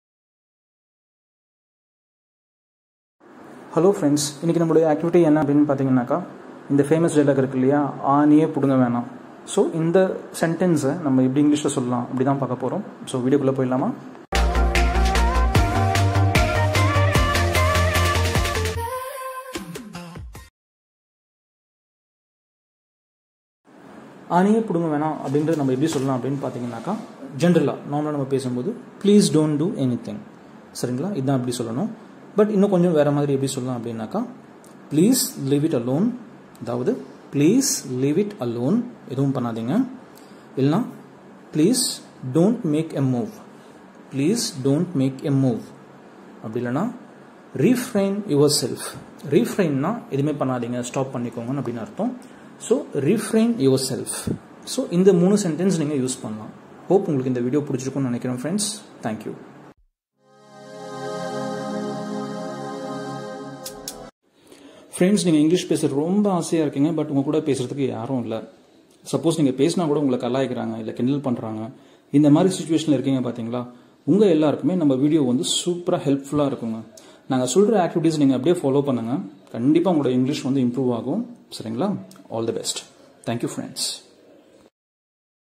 Vocabulary | How to say " please don't do anything " | Spoken English in Tamil
Spoken English in Tamil |English speaking tricks  | Speak English fluently.

In this video,  I have explained how to say "please don't do anything "

Good morning friends, I wish everyone to speak English as early as possible.

In your busy schedule, you may not have time to practice English, so its my small and sincere effort to make you practice English.